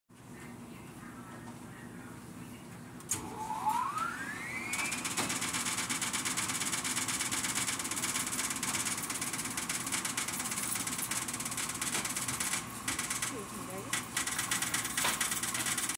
Wascomat W630 Washing Machine. Unbalanced 3rd Spin With Sudslock.
Those Are Not My Clothes.